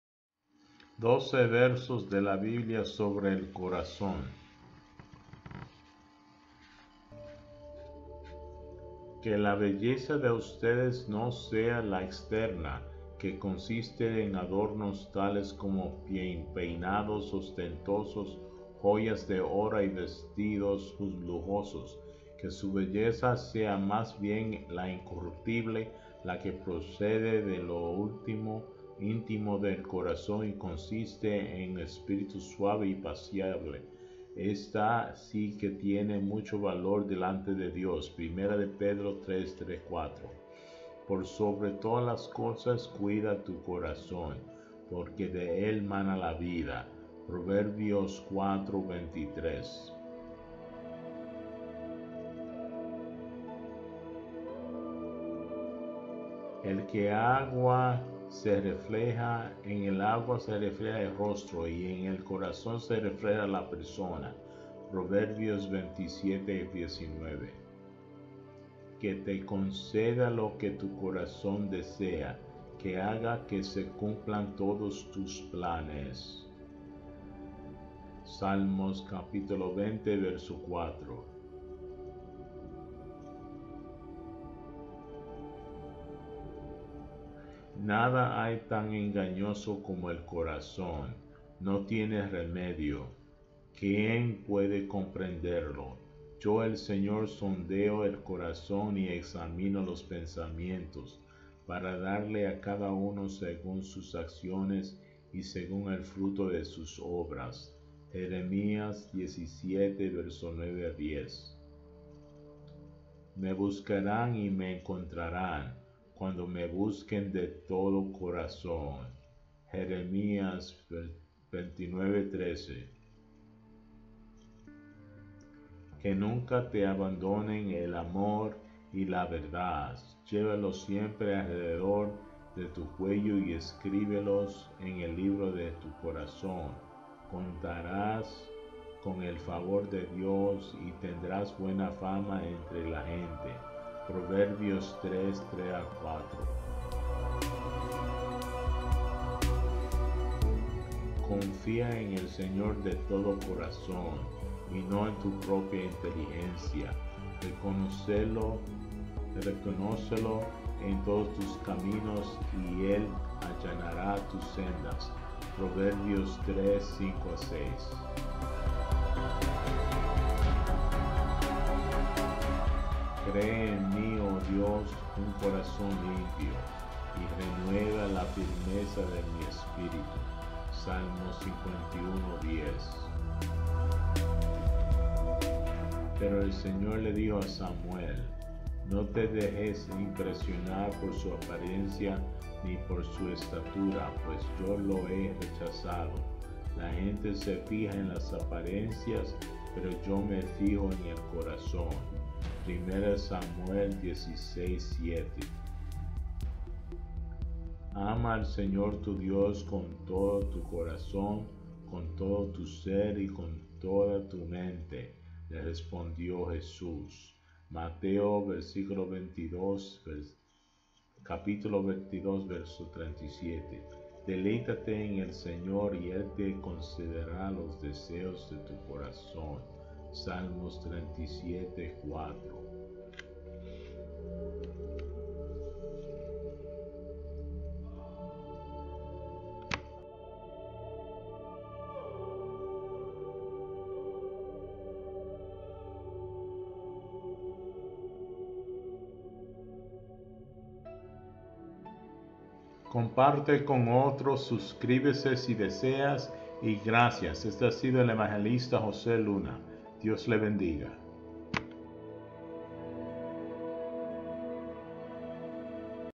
Poderosa Palabra de Dios Sobre El Corazón - Jesus te ama, orale!
El corazón tiene que ver con las decisiones que hacemos junto con la mente.  Tengamos cuidado de como llegamos a esas decisiones.  Hay algunas personas que dicen que no tienen que preocuparse por ir al infierno porque no hacen nada malo.  Dios dijo que fuera santo porque yo soy santo.  Todos pecamos y necesitamos arrepentimiento para tener razón con Jesús.  Oren a Jehová, a Jesús y al Espíritu Santo por la santidad.